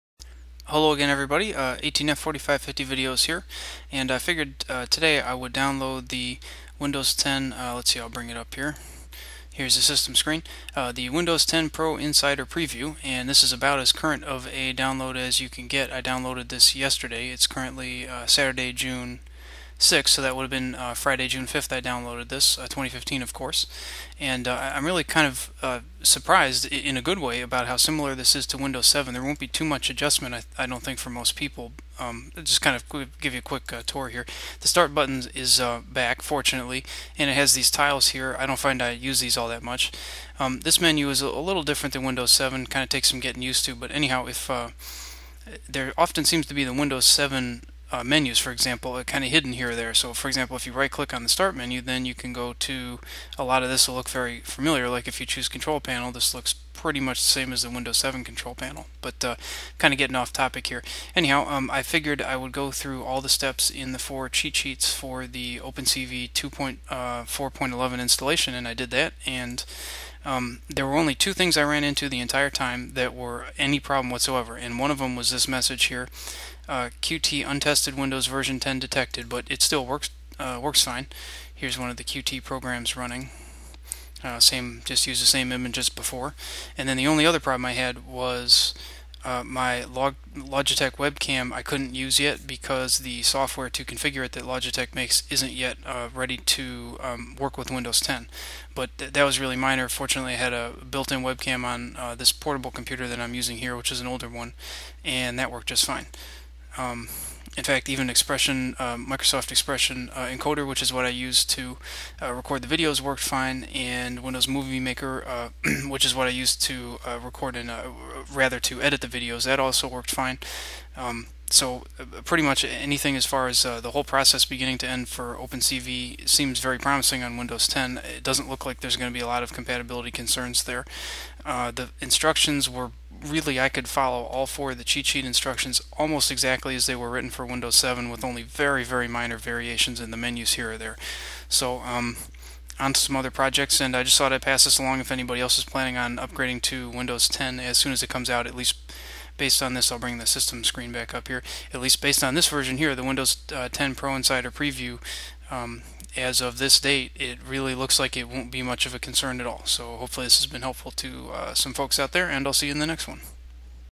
OpenCV 2.4.11 Installation Guide also works on Windows 10 Pro Insider Preview !!
Great news!  It looks like we (i.e. OpenCV users / enthusiasts) will have few if any change over compatibility concerns when transitioning from Windows 7 to Windows 10!

Please note this is 100% based on the Windows 10 "Pro Insider Preview" as of early June, I doubt the retail release (due in about 2 months) will change much but we can't be certain of this yet.

If you have not seen the OpenCV 2.4.11 Installation Guide I was referring to, please go to: